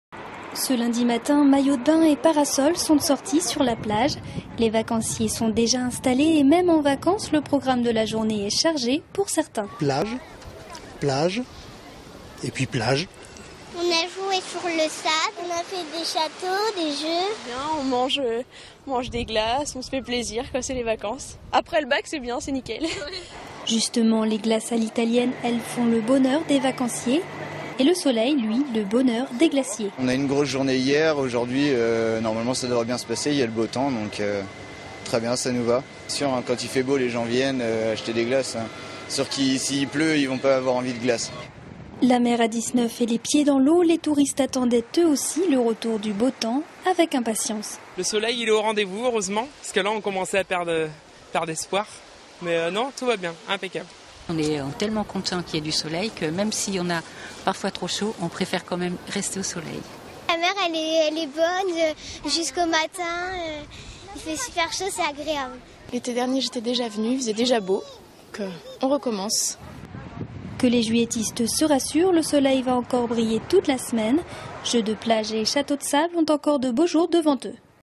Soleil : Les juilletistes ont le sourire (Vendée)
Sites webs/blogs, copiez cette vidéo avec le bouton "Intégrer" !
 Aux Sables-d'Olonne, les  vacanciers retrouvent enfin le sourire avec l'été qui est bien là.